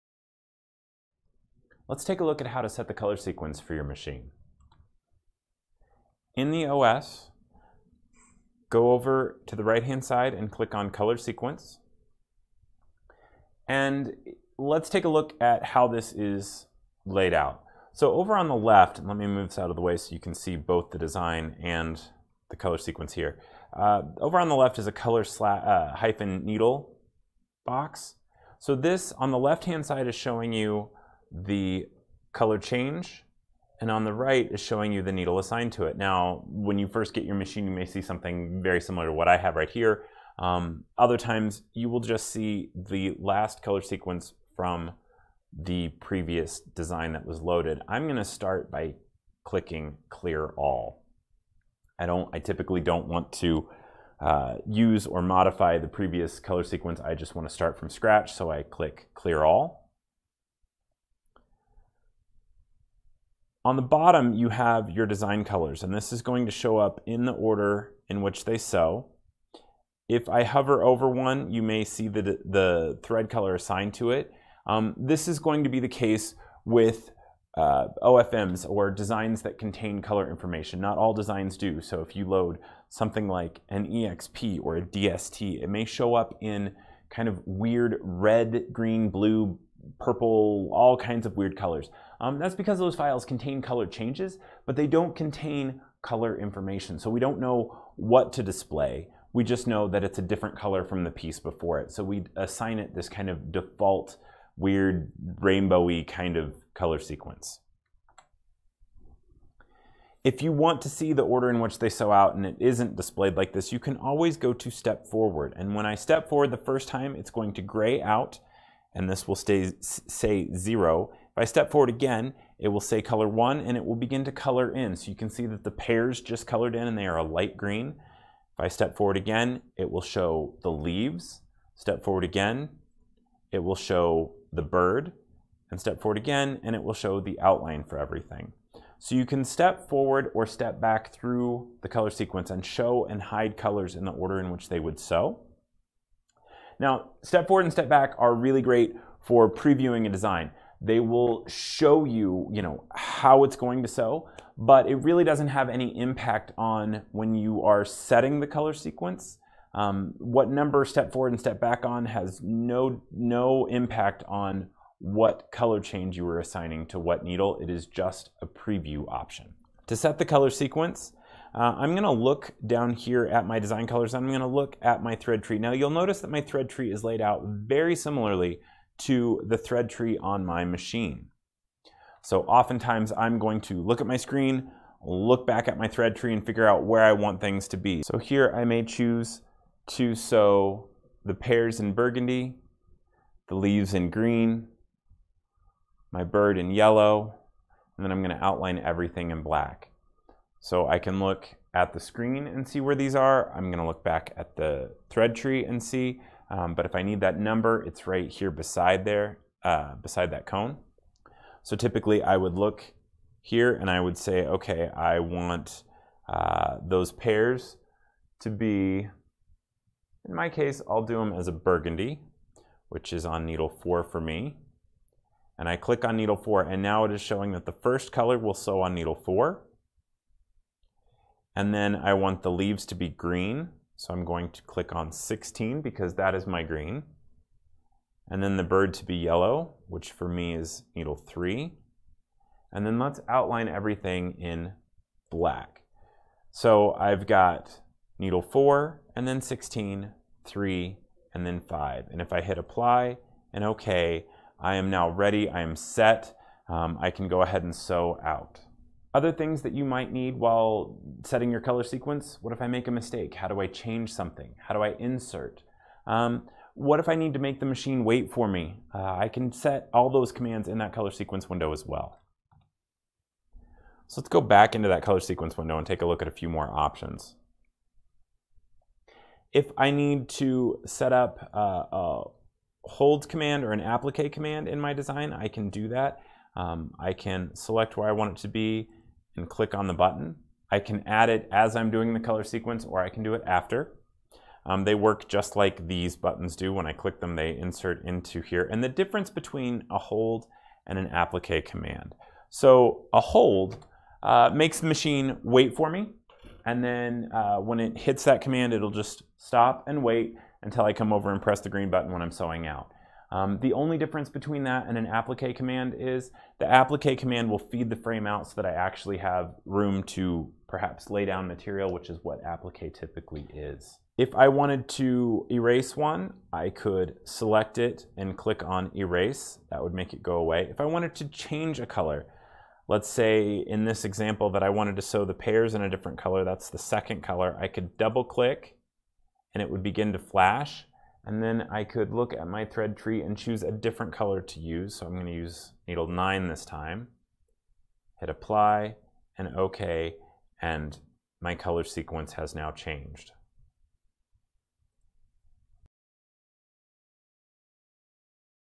Setting the Color Sequence (Advanced Interface)
Set the color sequence for your design in the software.